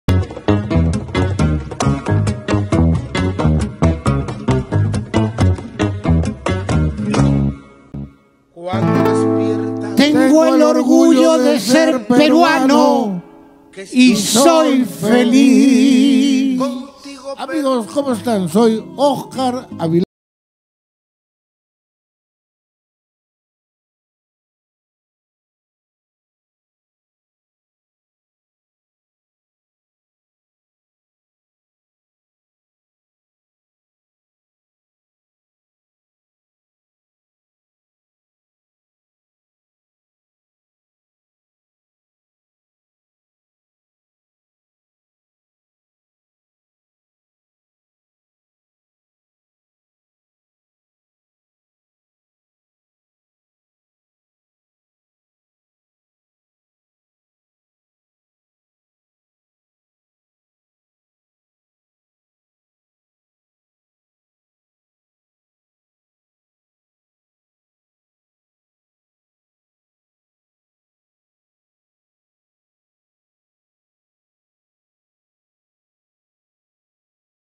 LOS AVILÉS @ DROM NY 28 DE JULIO 2022 7PM
Celebra fiestras patrias peruanas en New York con LOS AVILÉS @ DROM
Una de las familias musicales más emblemáticas e importantes del Perú y Latinoamerica llega este 28 de Julio, 7PM  a New York, la Capital de la música y del mundo "Los Aviles" presentando su espectáculo
" Viva el Perú " festejando el aniversario patrio en DROM,  el lugar de las estrellas.